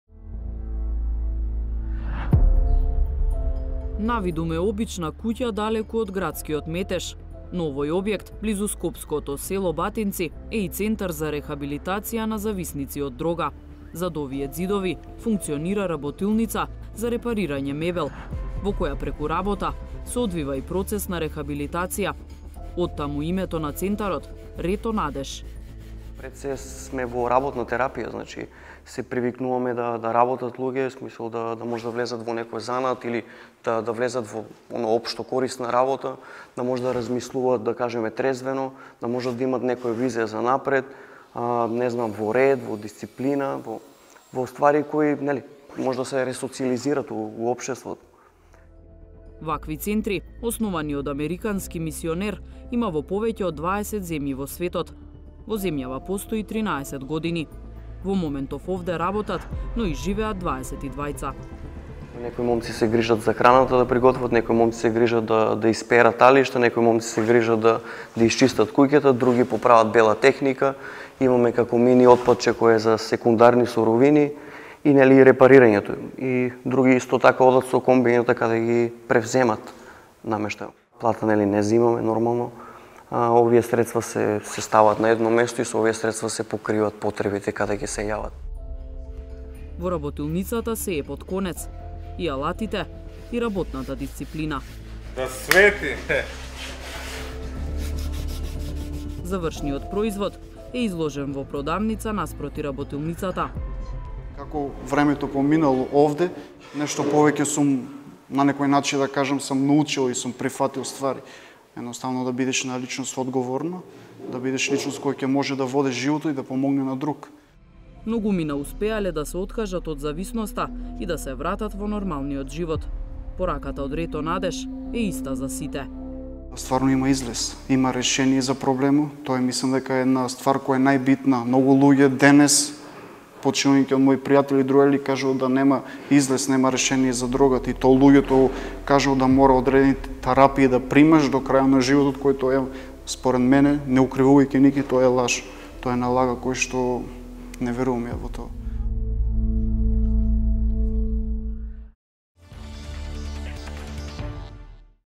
„Рето Надеж“ - работилницата за мебел која е и центар за рехабилитација на зависници од дрога.
Однадвор е обична куќа. Но одвнатре овој објект е работилница за мебел каде се рехабилитираат зависници од дрога.

Сторија за центарот „Рето Надеж“ во скопско Батинци.